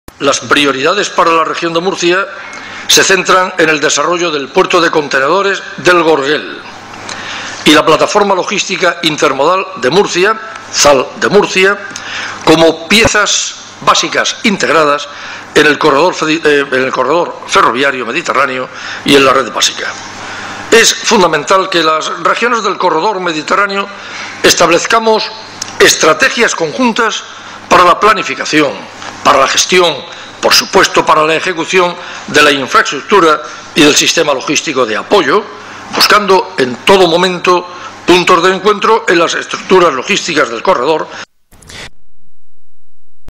Valcárcel: 'El Gorguel y la Plataforma Logística, prioridades en el Corredor Mediterráneo'
El presidente de la Comunidad, Ramón Luis Valcárcel, afirmó que en relación al Corredor Mediterráneo, "las prioridades para la Región de Murcia se centran en el desarrollo de Puerto de Contenedores del Gorguel y la Plataforma Logística Intermodal de Murcia". Valcárcel inauguró en Murcia la 'Jornada sobre las Redes Transeuropeas de Transporte. El Corredor Mediterráneo', un foro que cuenta con las aportaciones y planteamientos de los expertos europeos y técnicos de la Comisión y de la Administración regional que, según Valcárcel, "además de analizar la incidencia en nuestras regiones del nuevo Reglamento para el desarrollo de la Red Transeuropea de Transportes y del instrumento financiero asociado, debe servirnos también para esbozar puntos de encuentro de una estrategia común de transporte y logística para el desarrollo prioritario del Corredor Mediterráneo".